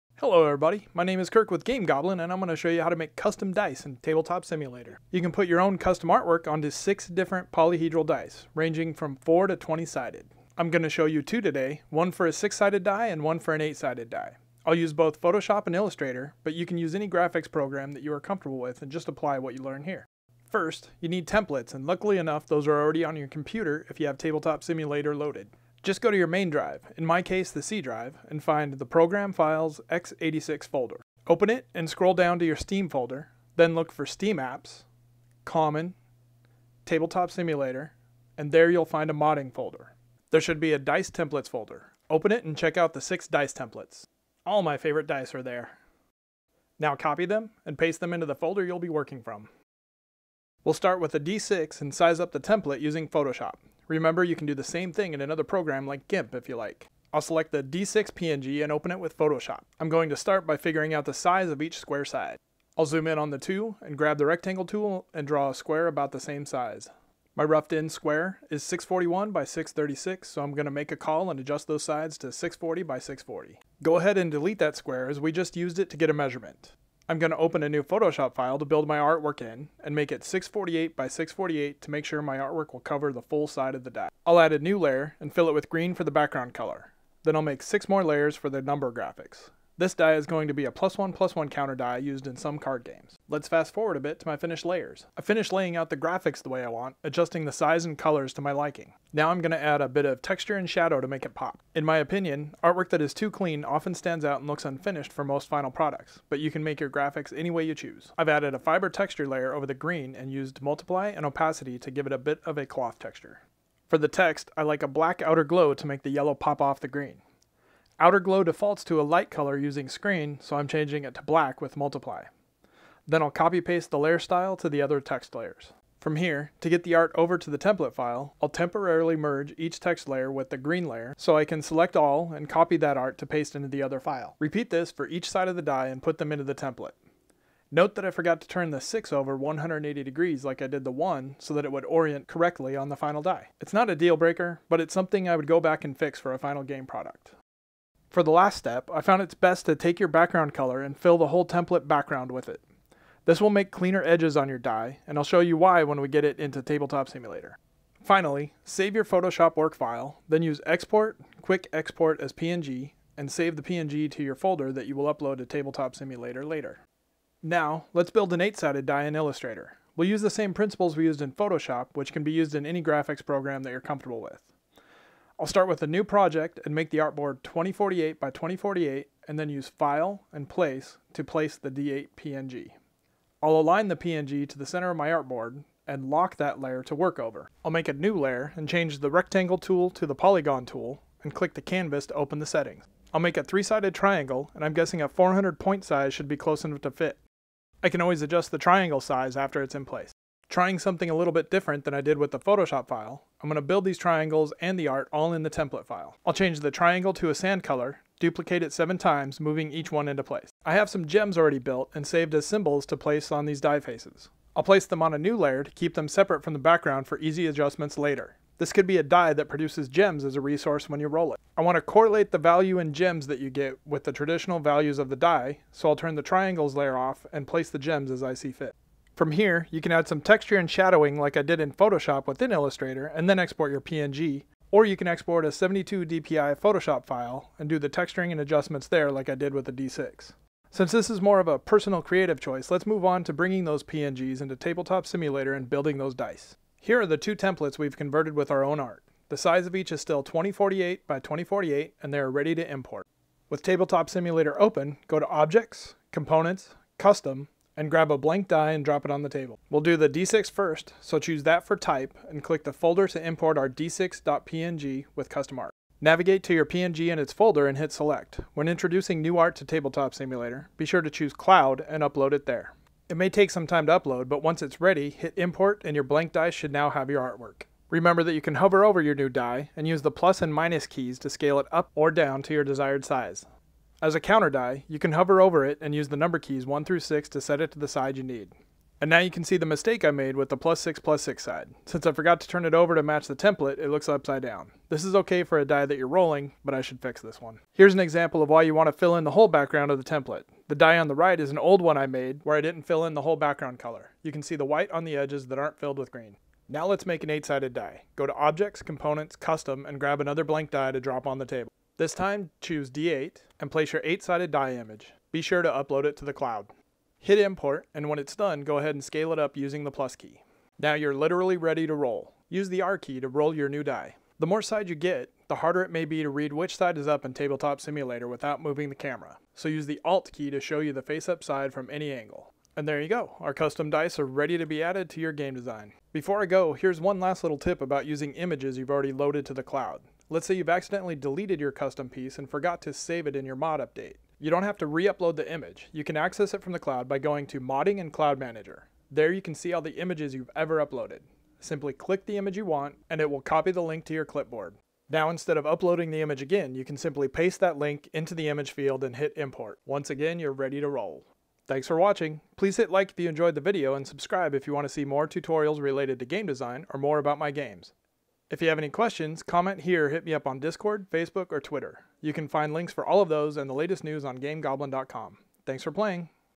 Tabletop Simulator - 03 Custom Dice Tutorial
Learn to make custom dice in Tabletop Simulator in under 6 minutes! I show you how in Photoshop and Illustrator, but you can apply the technique to ANY graphics program.

Find out more about my games on Tabletop Simulator and in print, or just stay up to date with my latest tutorials here: